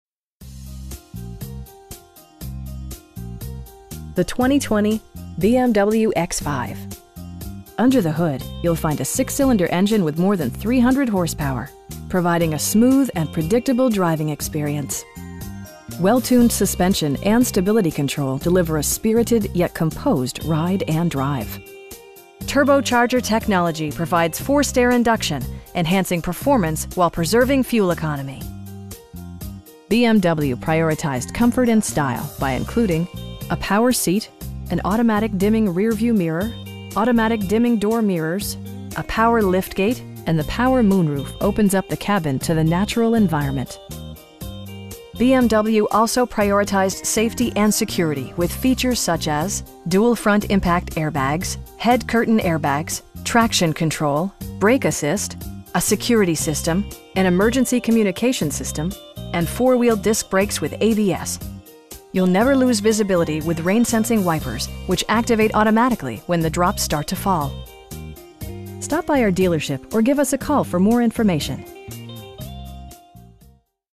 2020 BMW X5 xDrive40i in Richmond, VA 23294
Richmond BMW
8710 Broad St.

The 2020 BMW X5 Under the hood you'll find a 6 cylinder engine with more than 300 horsepower, providing a smooth and predictable driving experience. Well tuned suspension and stability control deliver a spirited, yet composed, ride and drive Turbocharger technology provides forced air induction, enhancing performance while preserving fuel economy. BMW prioritized comfort and style by including: a power seat, an automatic dimming rear-view mirror, automatic dimming door mirrors, tilt steering wheel, a power liftgate, cruise control, and the power moon roof opens up the cabin to the natural environment. BMW also prioritized safety and security with features such as: dual front impact airbags, head curtain airbags, traction control, brake assist, a security system, an emergency communication system, and 4 wheel disc brakes with ABS. You'll never lose visibility with rain sensing wipers, which activate automatically when the drops start to fall. We know that you have high expectations, and we enjoy the challenge of meeting and exceeding them! Stop by our dealership or give us a call for more information.